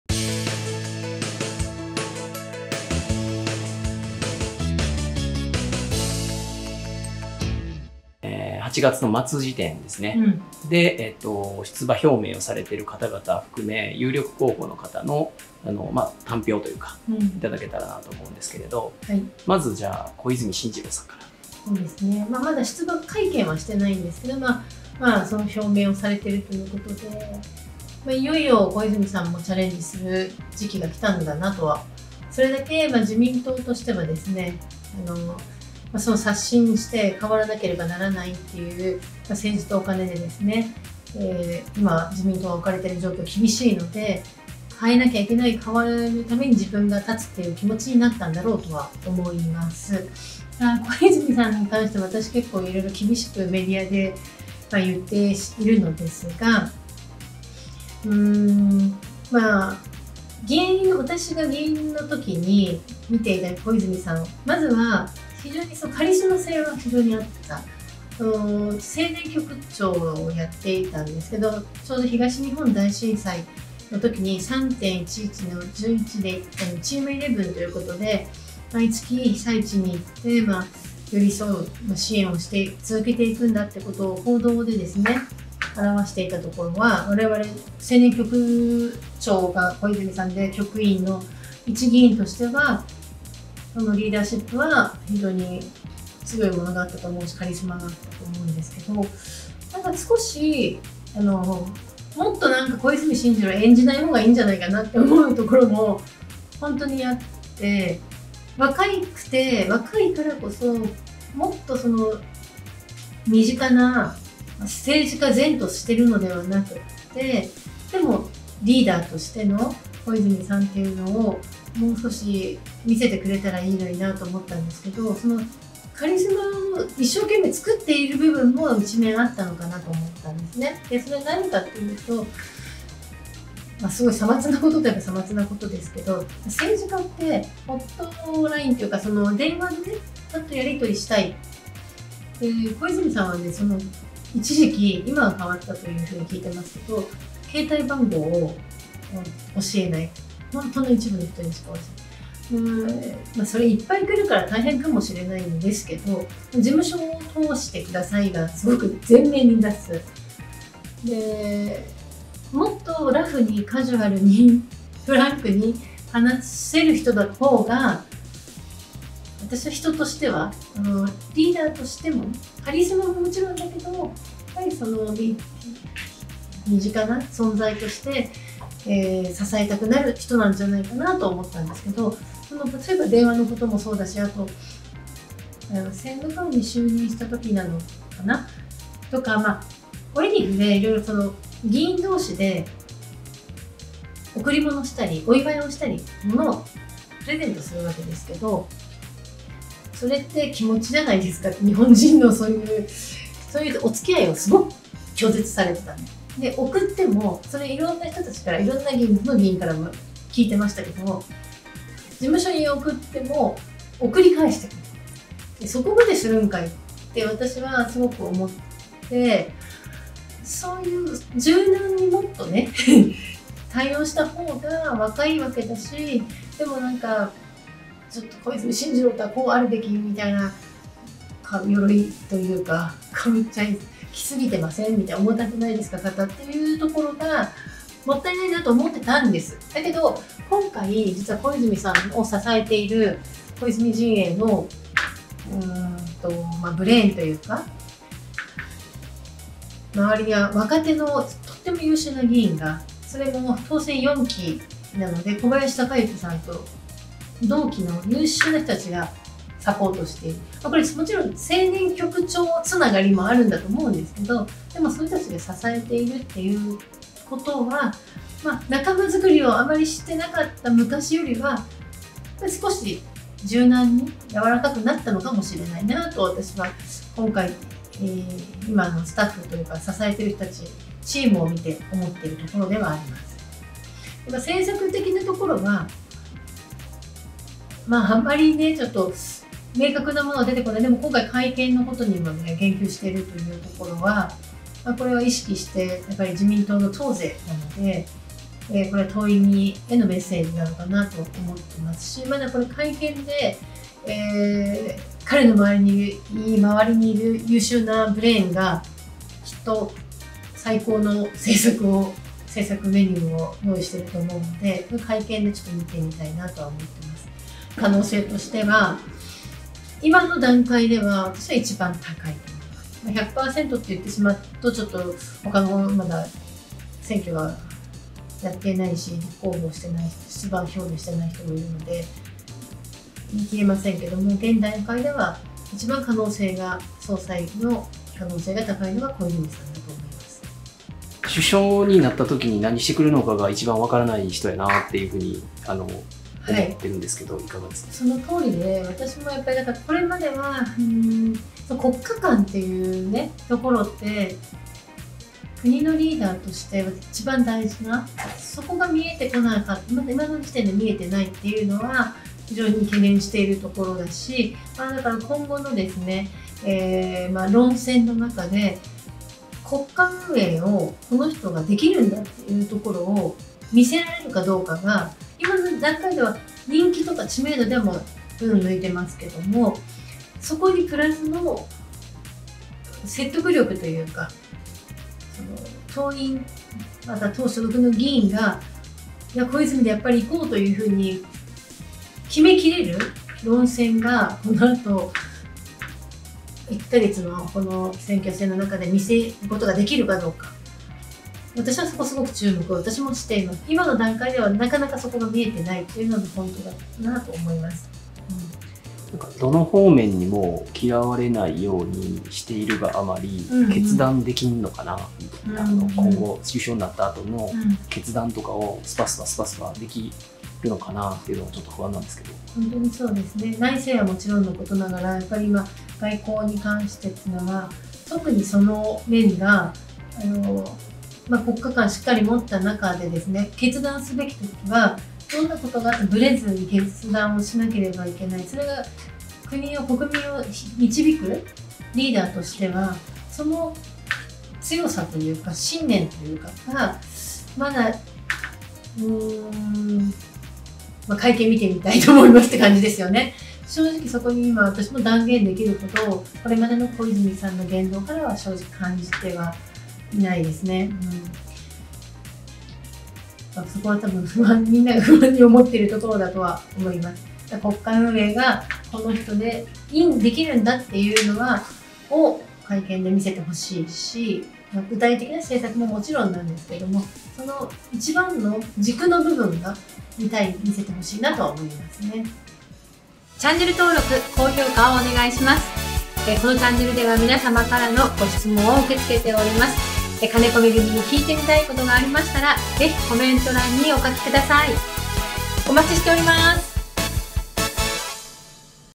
候補者乱立の自民党総裁選！ 注目される小泉進次郎氏を金子さんが斬る！
金子恵美チャンネル
第44回は引き続き自民党総裁選を解説。
動向が注目される小泉進次郎元環境相を
金子恵美が徹底分析します！

金子恵美/Kaneko Megumi